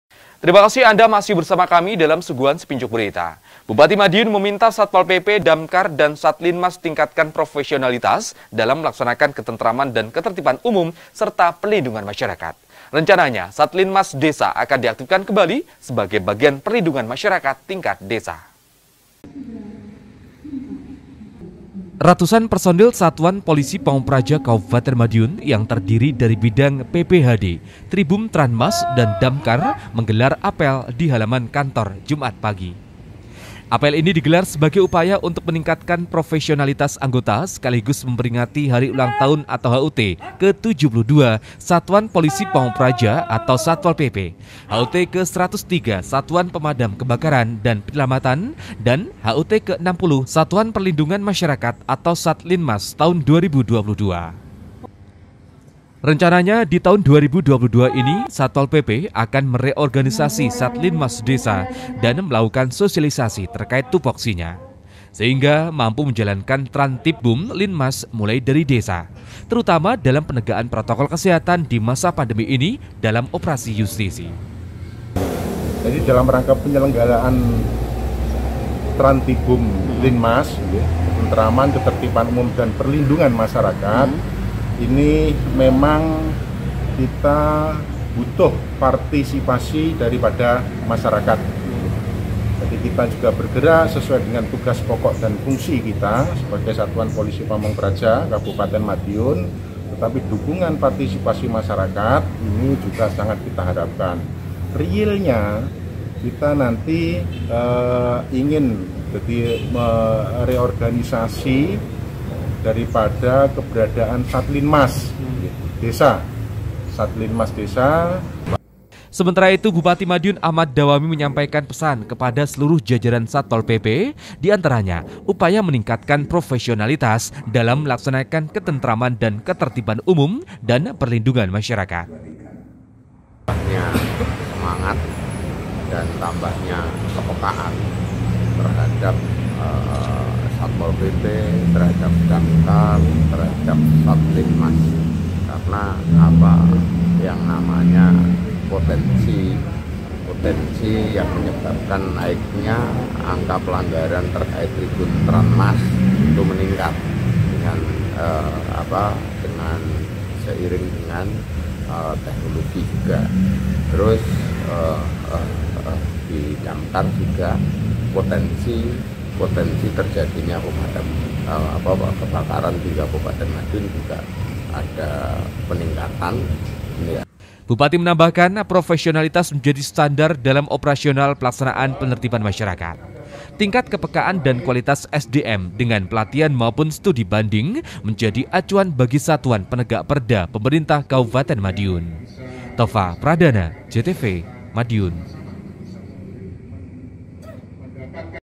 KAB MADIUN - Bupati Minta SATPOL PP Tingkatkan Profesionalitas Tribuntanmas Tingkat Desa
Bupati madiun minta satpol pp, damkar dan satlinmas tingkatkan profesionalitas dalam melaksanakan ketentraman dan ketetiban umun serta perlindubgan masyarakat. Rencananya satlinmas desa akan diaktifkan kembali sebagai bagian perlindungan masyarakat tingkat desa .